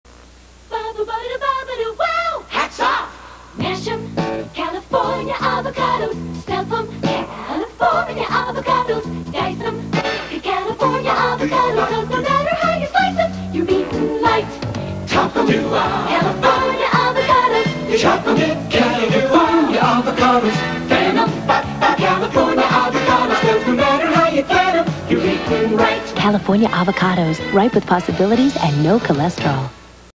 1987 California Avocado Commision "California Avocados" TV Commercial
This commercial aired on January 23rd, 1987. It was shown on during the broadcast of "Dallas" on Channel 5 KPIX.